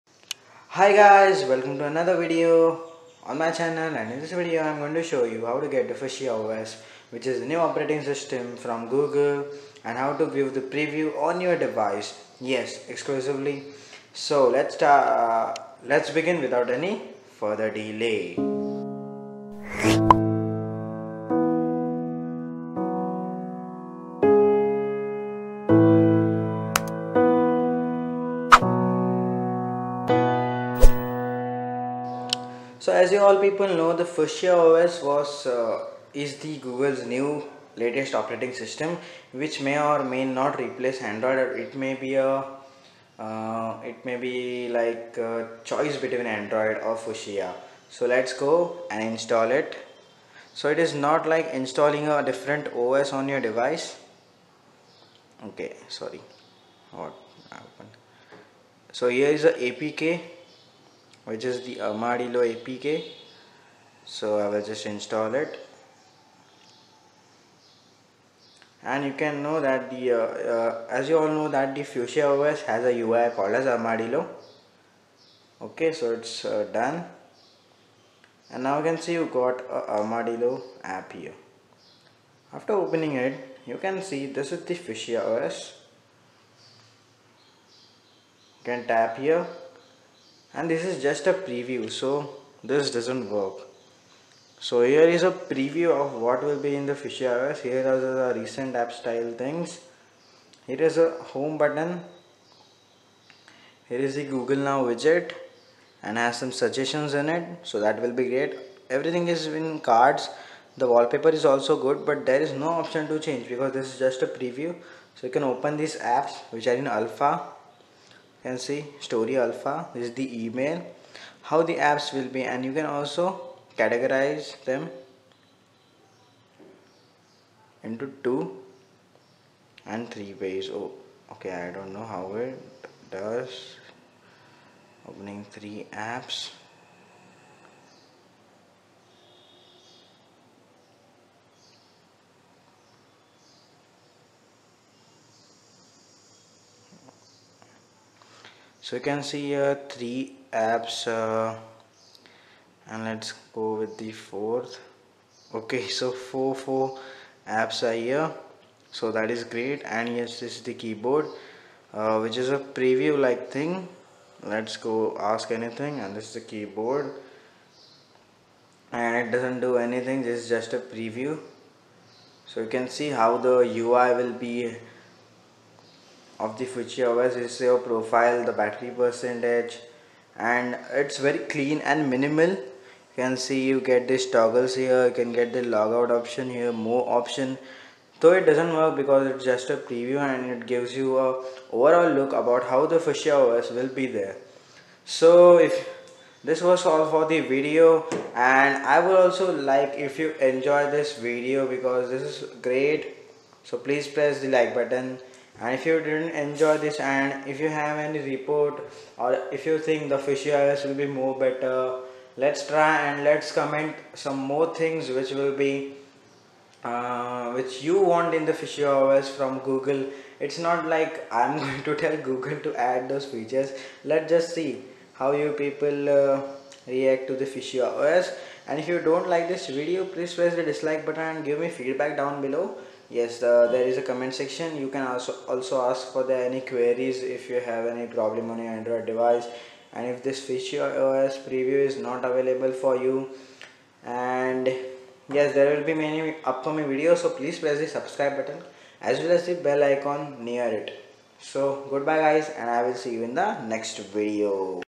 Google's New Fuchsia OS Preview On Android! [Without ROOT]
In this video, I'm presenting and sharing you the preview of the new operating system being developed by google called as Fuchsia OS.

Experience the Fuchsia OS yourself! -